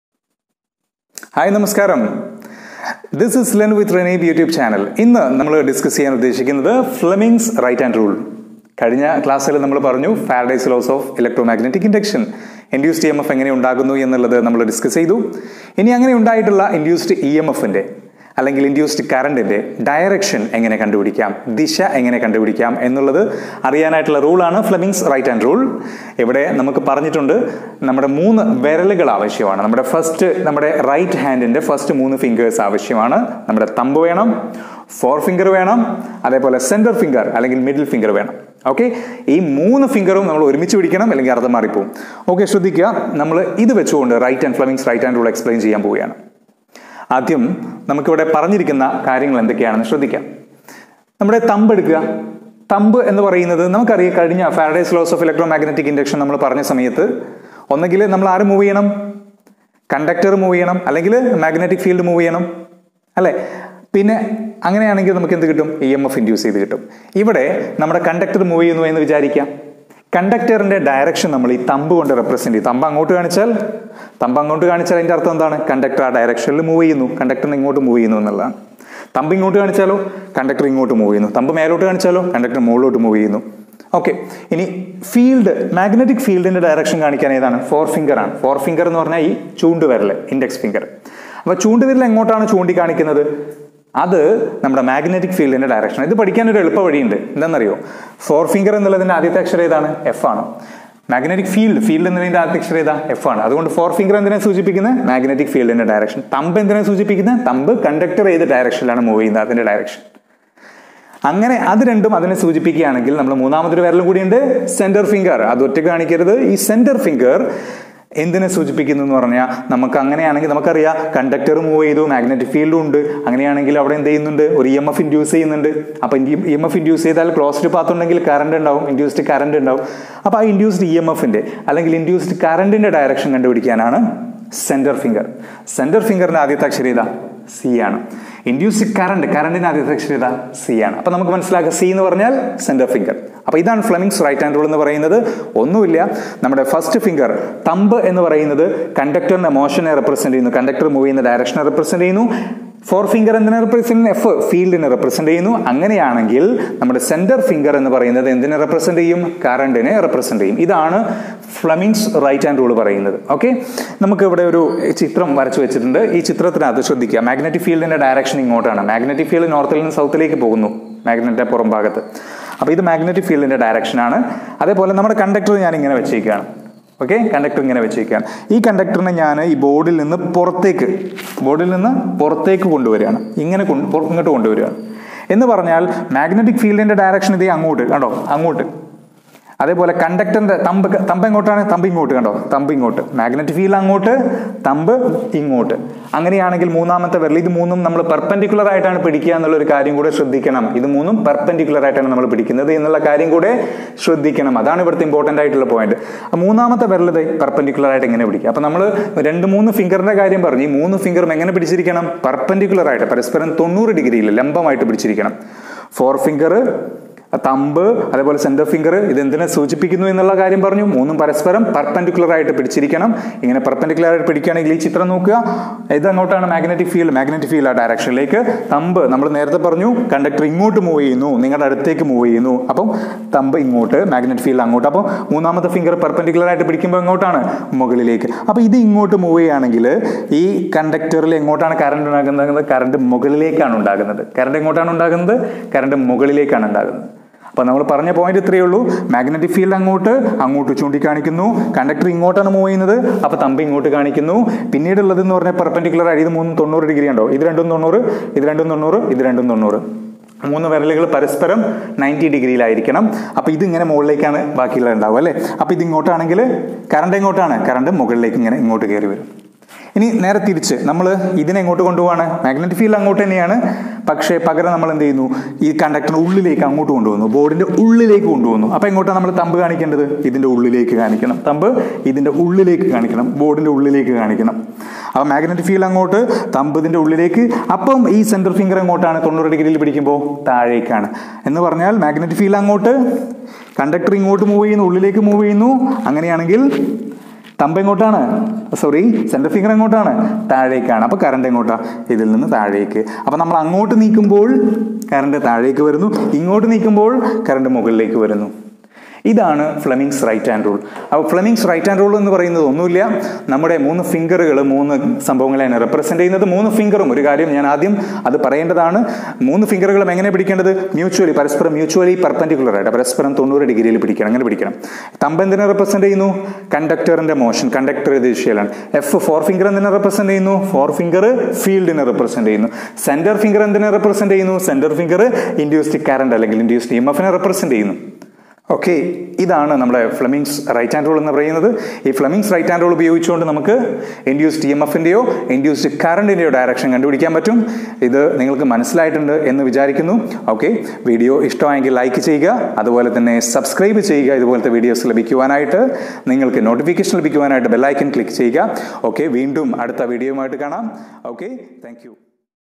Fleming's Right Hand Rule
Fleming's Right Hand Rule simple explanation.